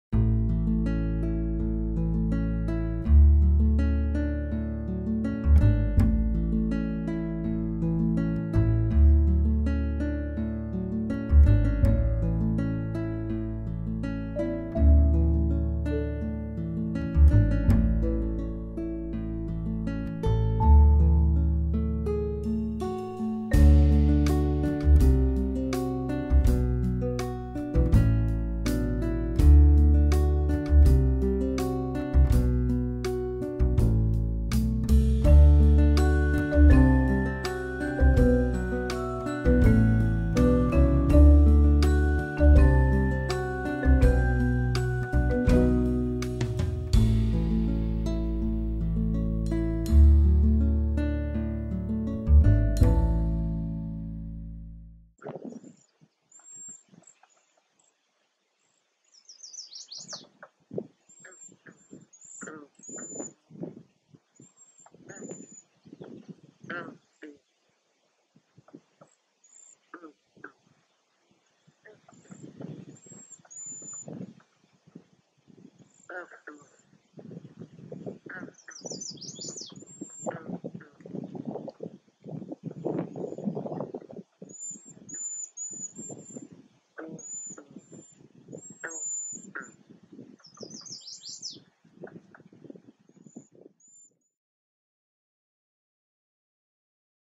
Jaseur d'Amérique - chant
Jaseur d’Amérique. On peut entendre son chant à la fin, accompagné du chant d’un autre oiseau et des grenouilles.